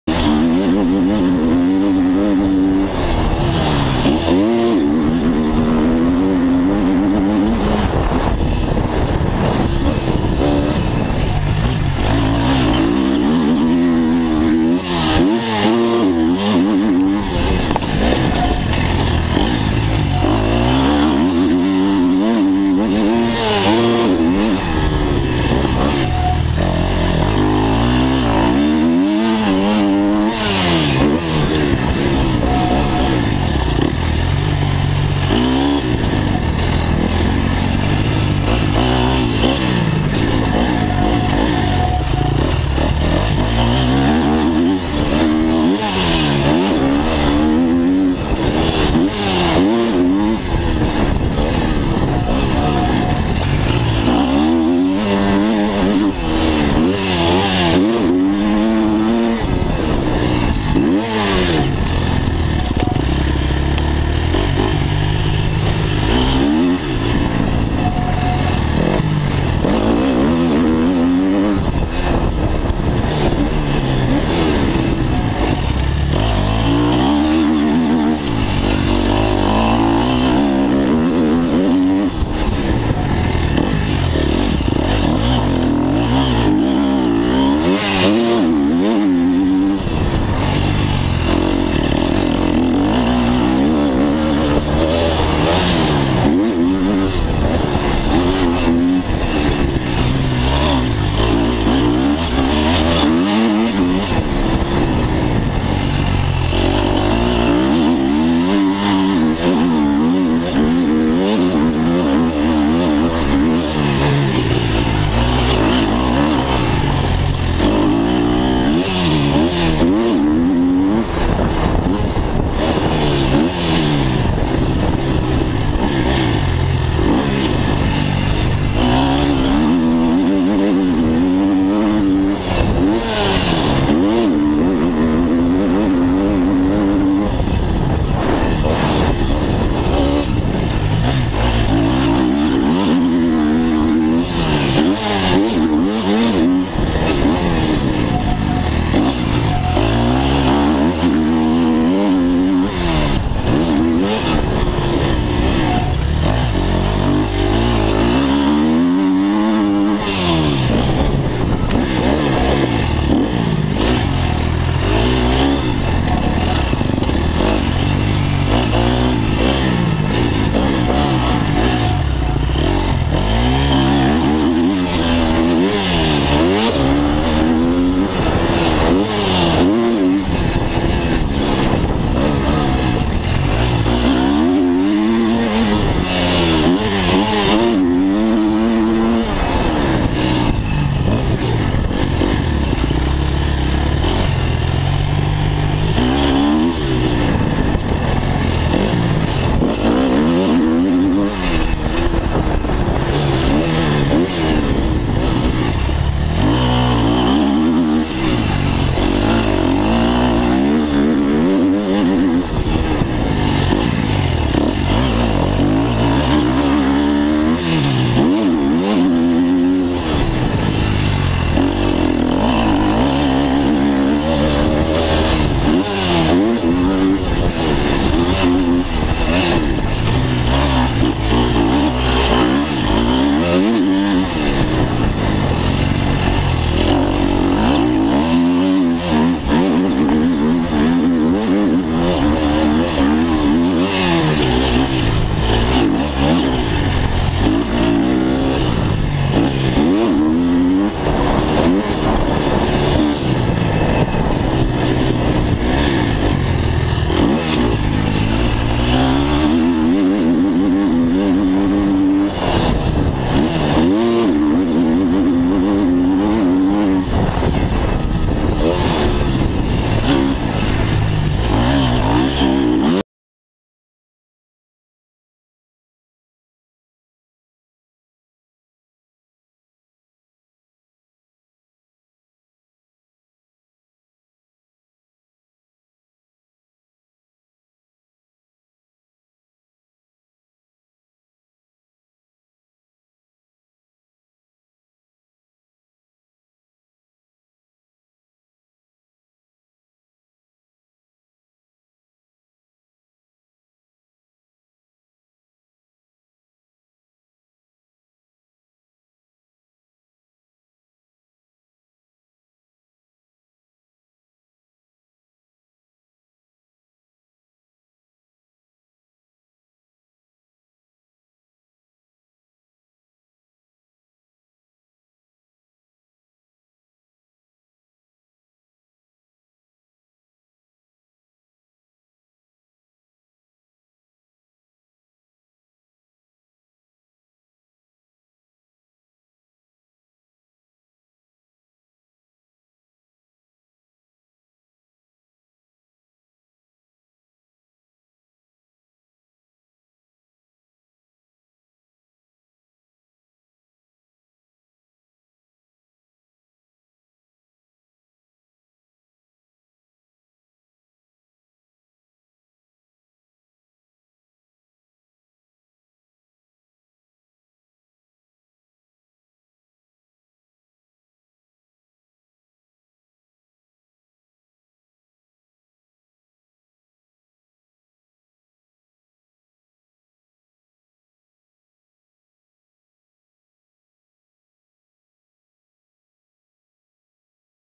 esanatoglia on board
2 giri on board sul fantastico tracciato di Esanatoglia in sella con Nico Di Mare #281